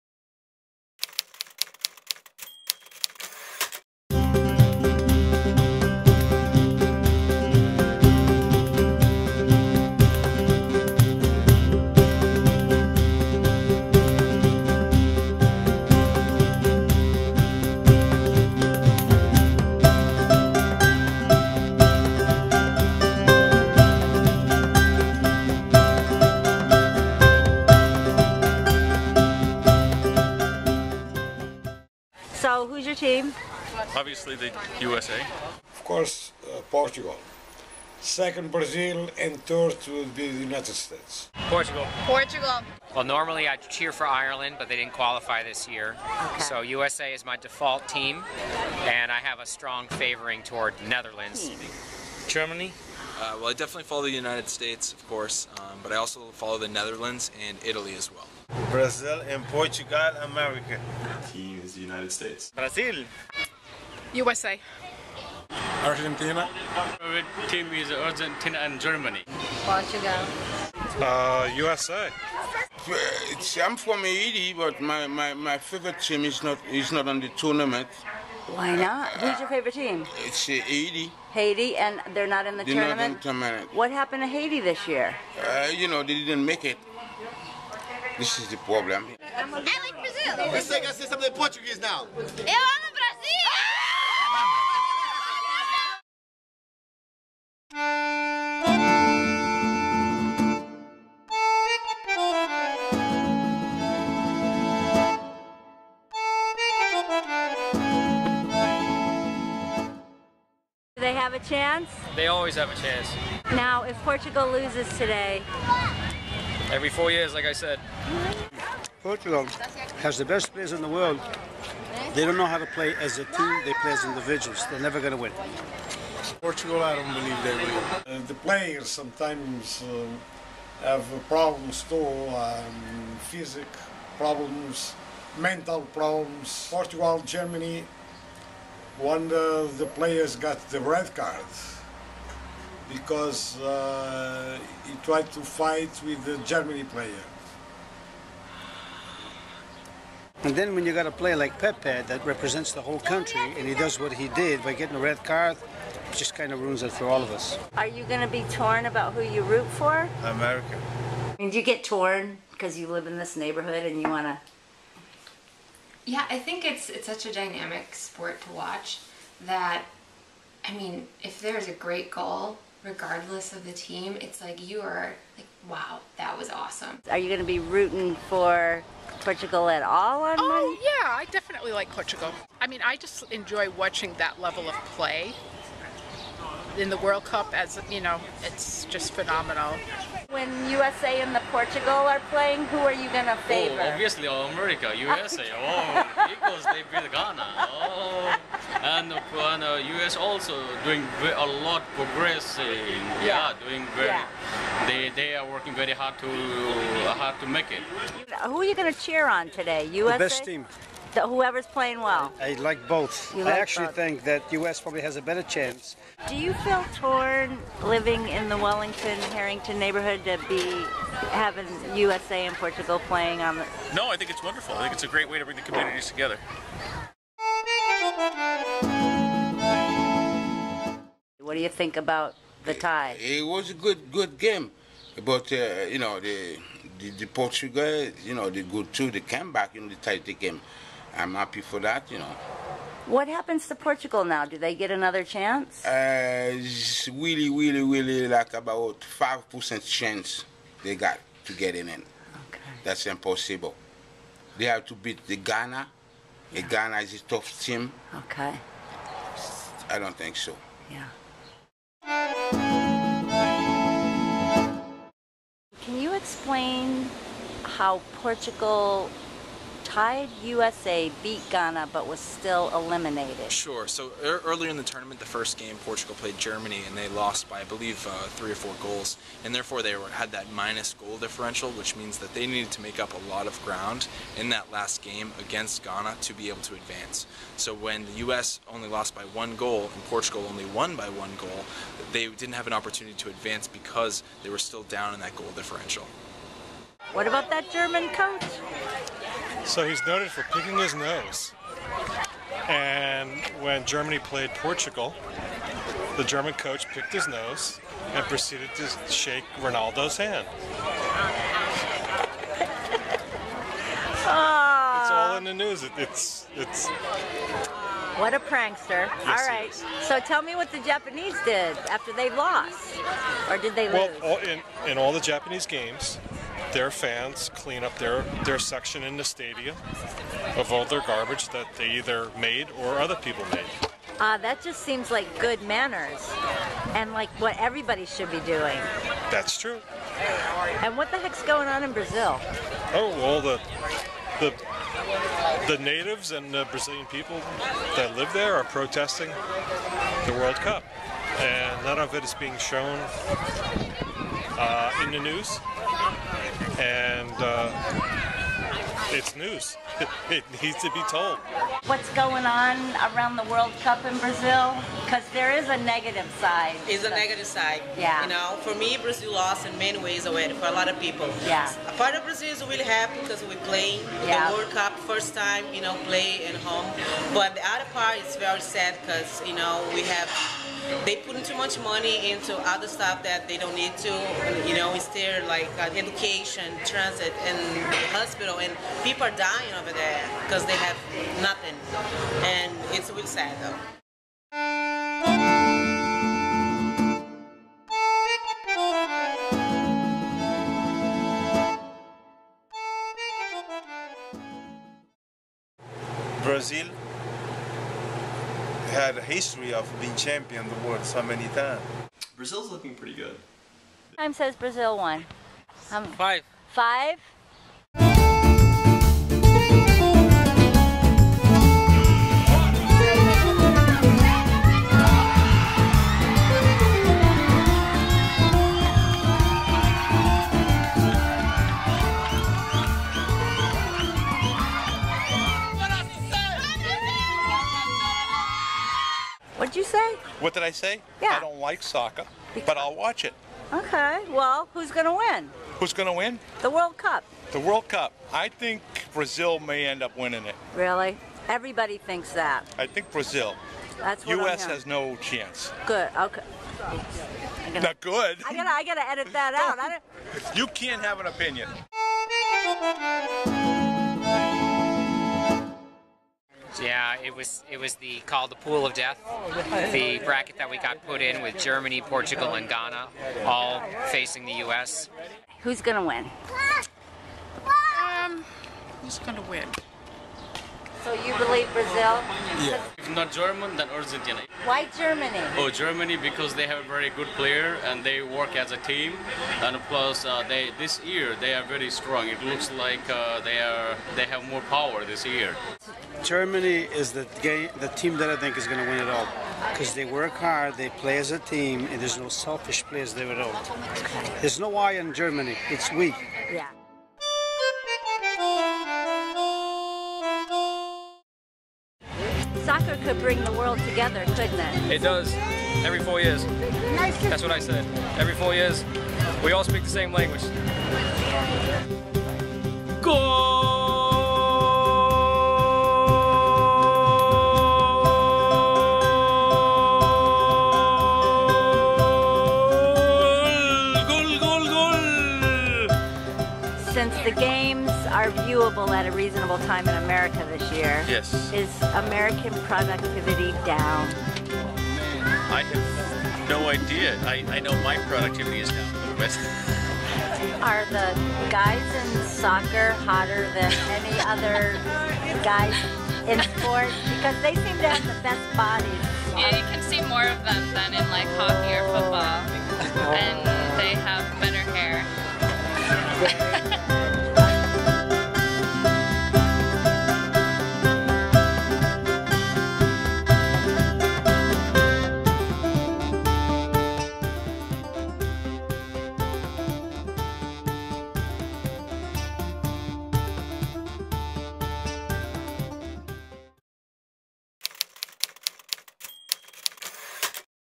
"The Beautiful Game"
Drinking from the World Cup of prognostication and illumination in the Wellington-Harrington neighborhood of Cambridge, Mass.